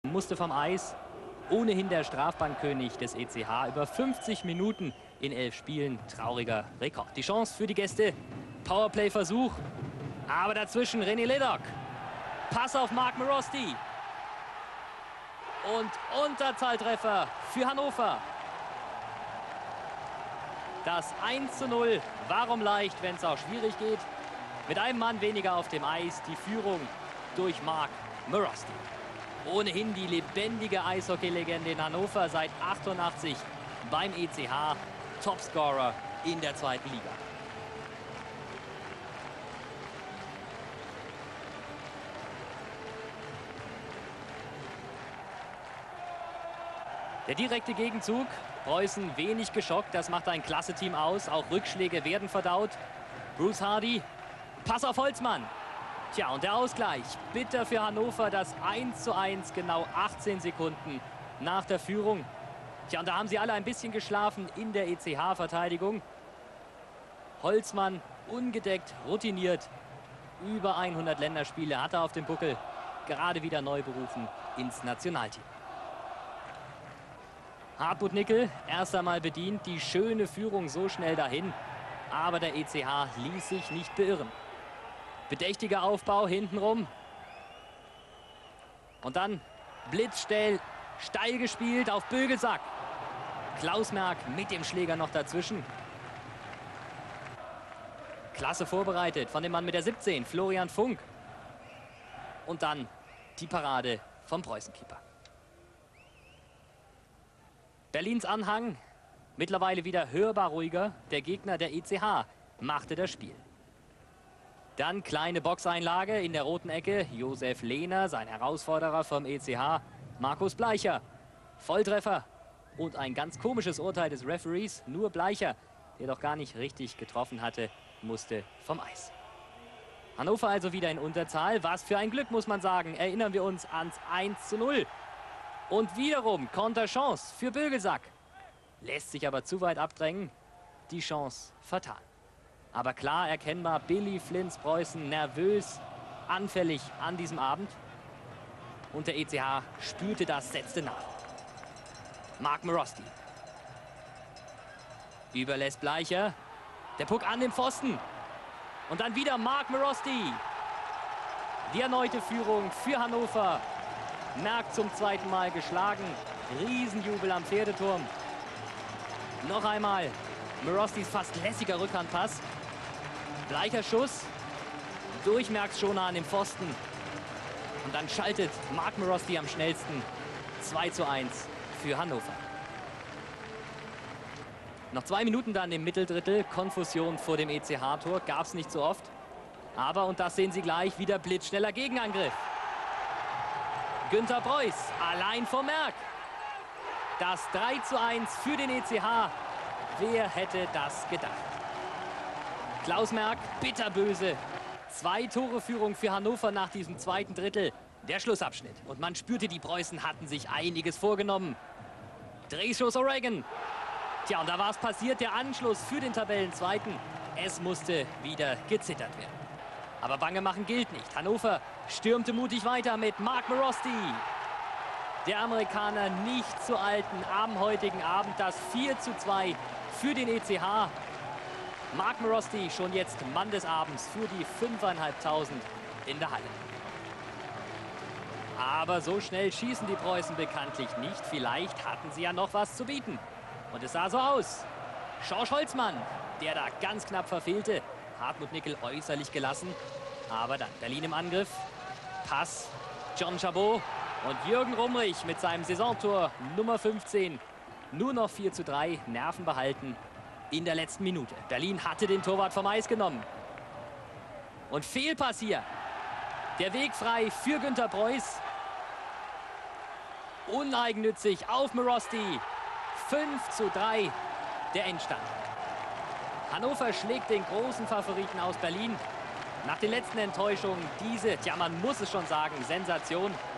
EC Hannover vs Preussen Berlin
vom 23.10.94 Ergebnis 5-3...ab 2.Drittel